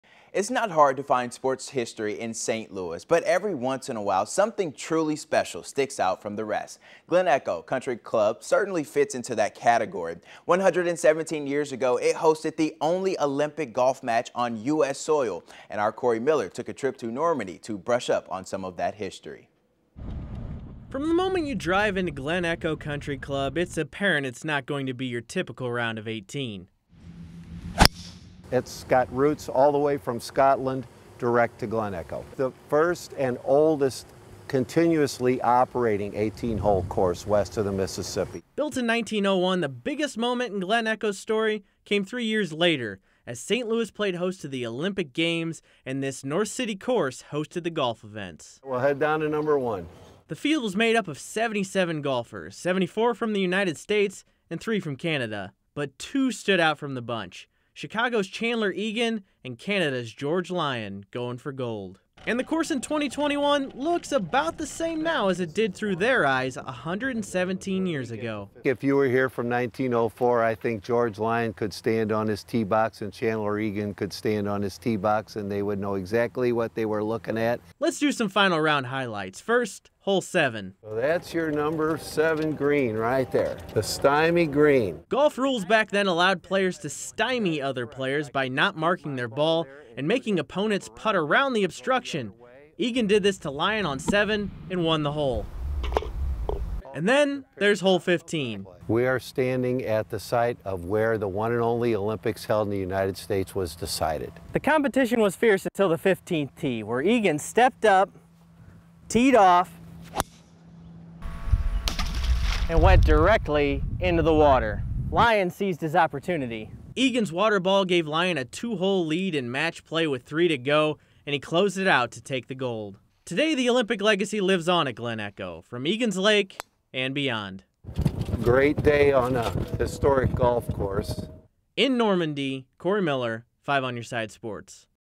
St. Louis Olympic legacy burns bright at Glen Echo Country Club
NORMANDY, Mo. - From the moment you turn into Glen Echo Country Club, it’s apparent it’s not going to be your typical round of 18 holes. It immediately feels special.

The oldest continuously-operating, 18-hole course west of the Mississippi River has roots that go all the way back to the birthplace of golf in Scotland.

Designed by the well-known Foulis brothers, Glen Echo opened for operation in 1901. But the biggest moment in the course's history came three years later in 1904.

St. Louis was hosting the Olympic Games, and Glen Echo got the nod to host the Olympic tournament. And like most everything else in the 1904 Olympics, the golf event was not without some issues.

Getting golfers from around the world to St. Louis proved tough, so the field of competitors was made up of 77 golfers, with 74 from the United States and three from Canada.

But two stood out from the bunch. Chicago’s Chandler Egan, and Canada's George Lyon separated from the pack as the men to beat for individual gold.